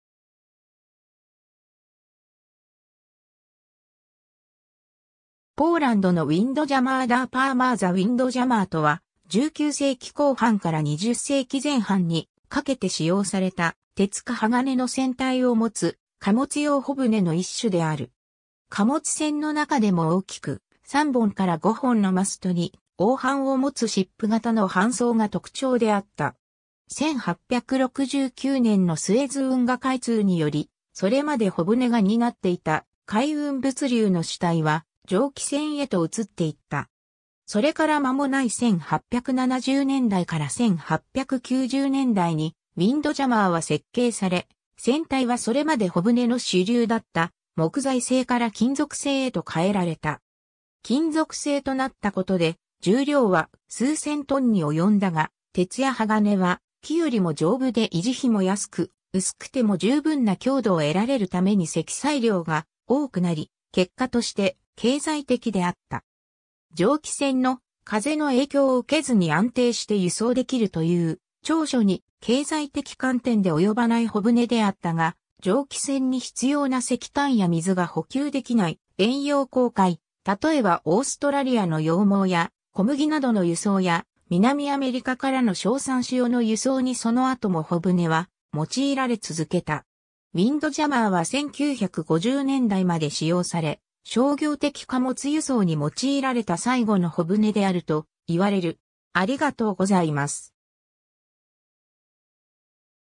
ウィンドジャマー
ポーランドのウィンドジャマー　Dar Pomorza ウィンドジャマー（Windjammer）とは、19世紀後半から20世紀前半にかけて使用された、鉄か鋼の船体を持つ貨物用帆船の一種である。
貨物船の中でも大きく、3本から5本のマストに横帆を持つシップ型の帆装が特徴であった。
 1869年のスエズ運河開通により、それまで帆船が担っていた海運物流の主体は蒸気船へと移っていった。
それから間もない1870年代から1890年代にウィンドジャマーは設計され、船体はそれまで帆船の主流だった木材製から金属製へと変えられた。
金属製となったことで重量は数千トンに及んだが、鉄や鋼は木よりも丈夫で維持費も安く、薄くても十分な強度を得られるために積載量が多くなり、結果として経済的であった。
 蒸気船の「風の影響を受けずに安定して輸送できる」という長所に経済的観点で及ばない帆船であったが、蒸気船に必要な石炭や水が補給できない遠洋航海、例えばオーストラリアの羊毛や小麦などの輸送や南アメリカからの硝酸塩の輸送にその後も帆船は用いられ続けた。
ウィンドジャマーは1950年代まで使用され、商業的貨物輸送に用いられた最後の帆船であるといわれる。